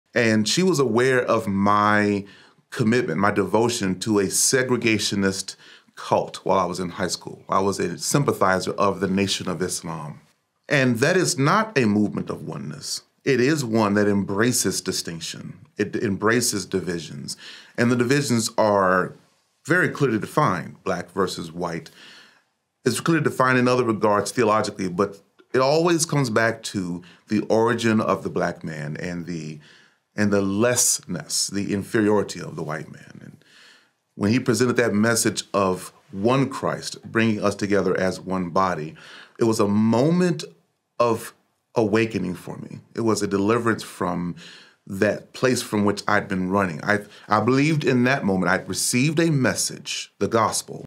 Finding my way out of a segregationist cult
American Evangelicalism is ablaze.

Doctrinal identity unites black and white evangelicals, but divisions along ethnic and cultural lines have long tarnished the movement's witness. With desegregation on the horizon, Martin Luther King Jr. said, "I've come to believe that we are integrating into a burning house." As with the country, if we hope to fully integrate the American evangelical church, we must do so as firefighters.

In A Burning House Video Study, Pastor Brandon Washington challenges American evangelicalism to embrace its historical commitment to orthodoxy and orthopraxy. Only then can it become a holy witness to humanity and embody shalom-- peace, justice, wholeness-- in the world. These are the inevitable fruits of espousing and preaching a comprehensive gospel message.